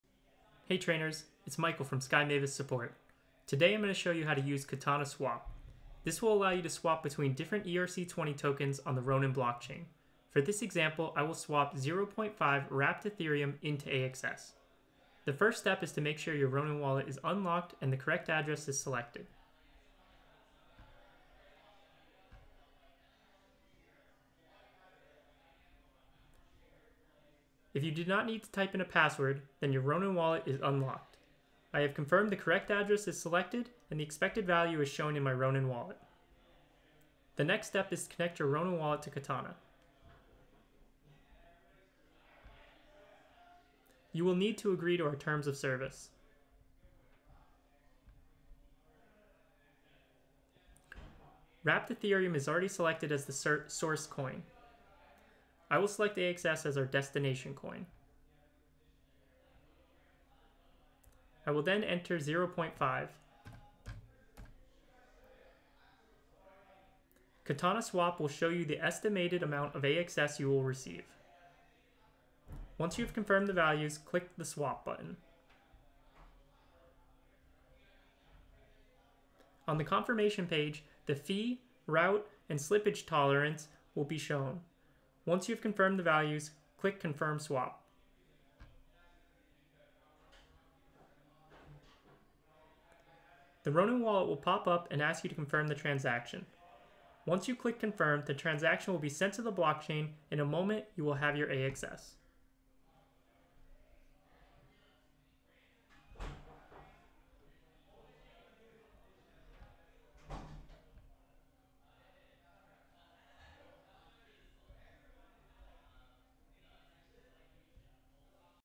Katana Swap Tutorial
How to swap tokens on Katana, the Ronin Dex, in an easy video tutorial.